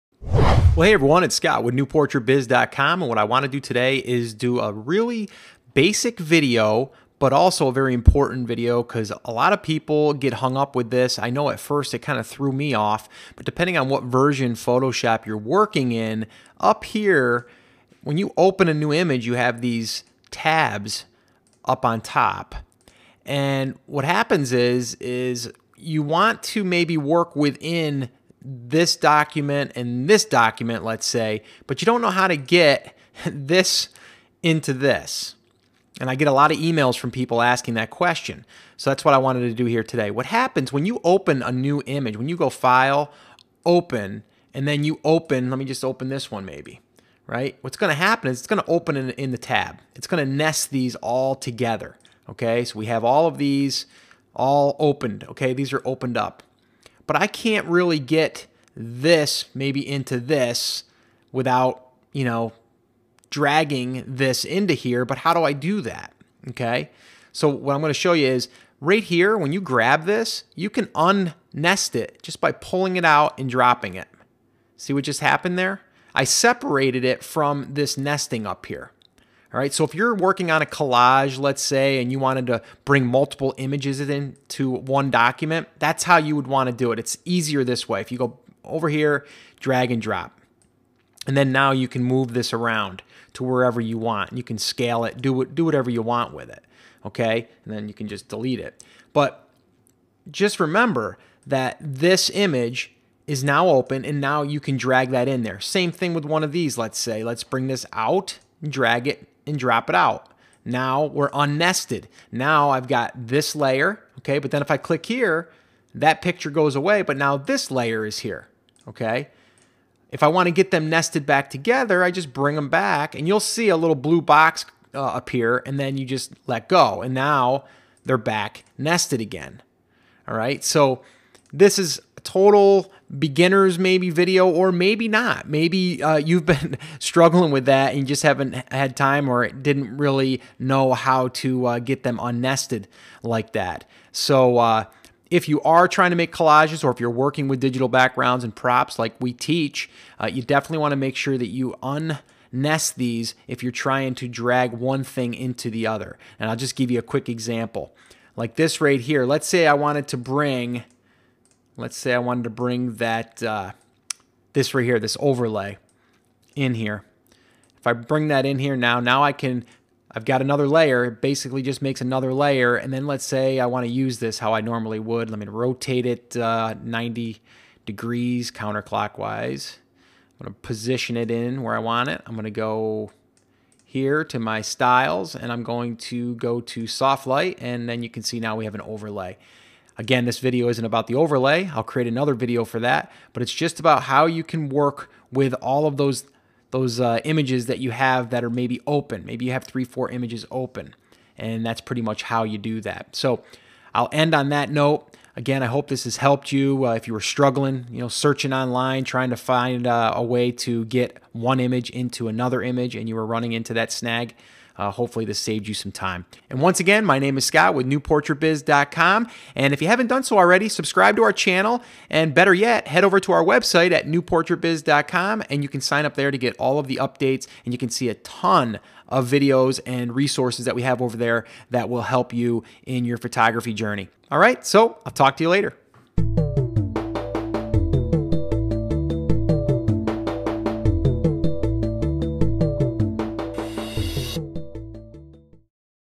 Photoshop Beginner Tutorial - Understanding Image Tabs For Multiple Pictures
- In this beginner Photoshop tutorial I'll be explaining how to work with image tabs and nesting. When opening an image in Photoshop it will create a new tab. You can separate these tabs by dragging them out from the nesting to create it's own tab.

Here's the link to the video I mentioned in this lesson for using overlays.